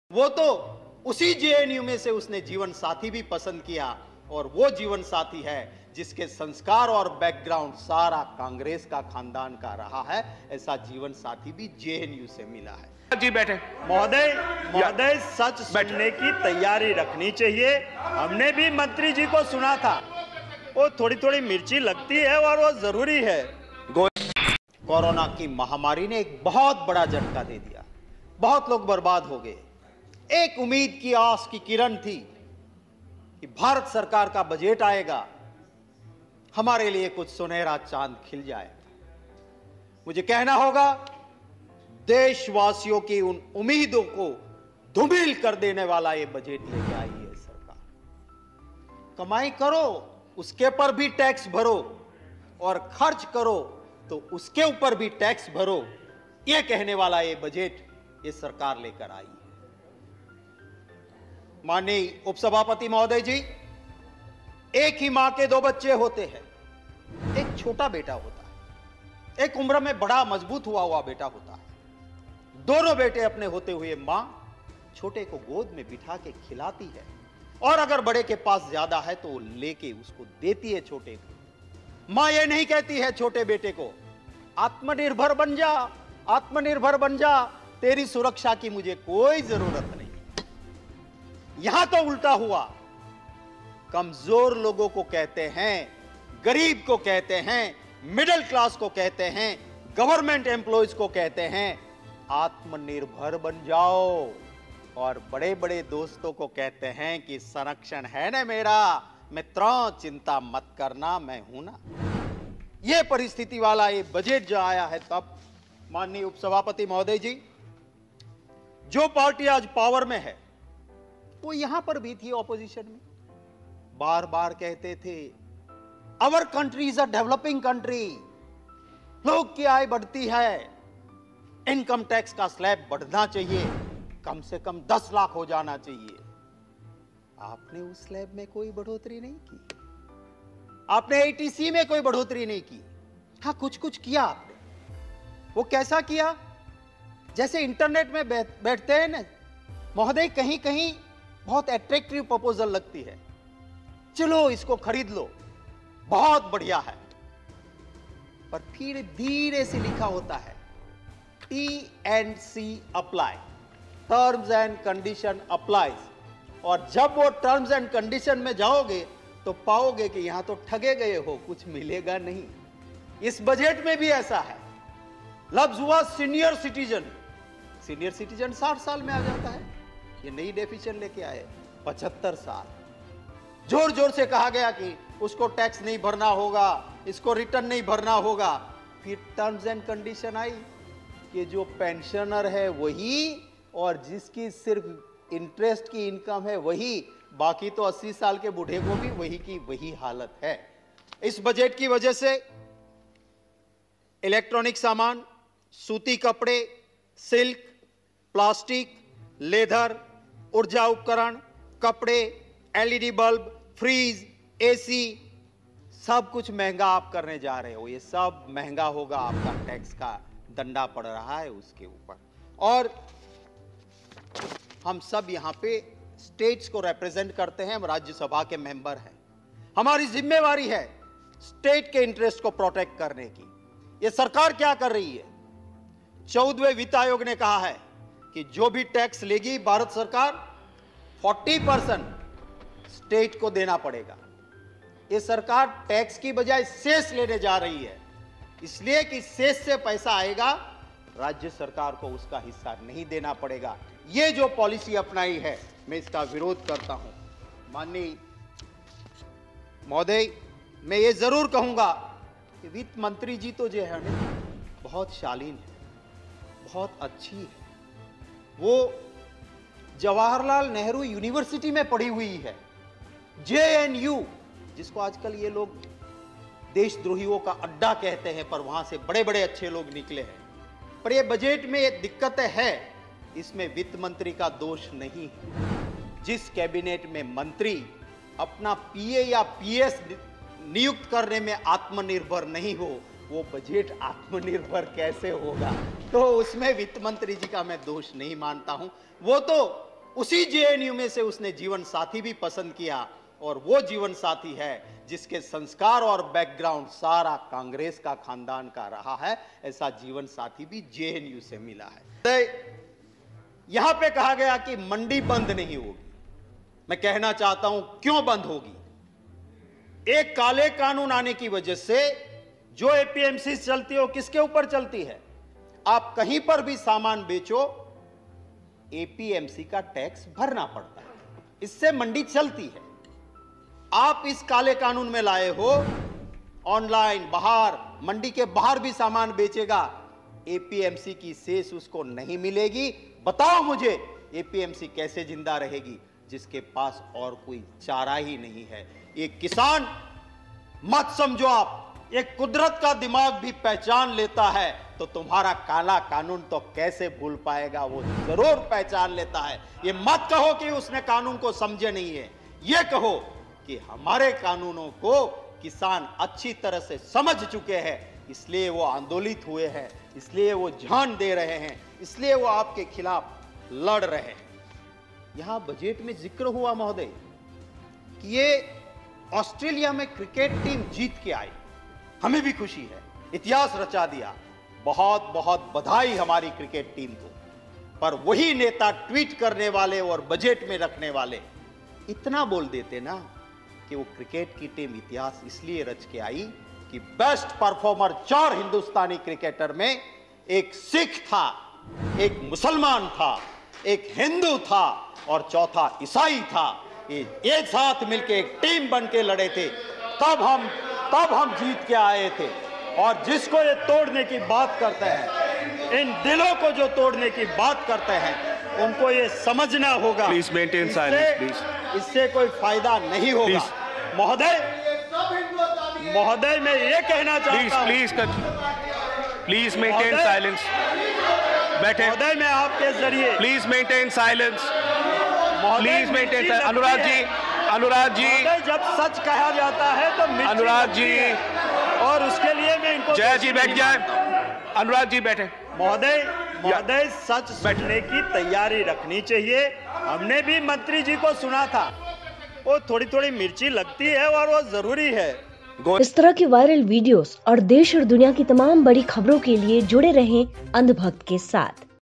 Shaktisinh Gohil ने Nirmala Sitharaman की लव मैरिज पर क्या कह डाला? | ANDHBHAKT
Targeting the Modi government on the farmer's Protest and the budget, Congress MP Shakti Singh Gohil gave a brilliant speech in Rajya Sabha. Shakti Singh Gohil also targeted Finance Minister Nirmala Sitharaman and Union Minister of State Anurag Thakur.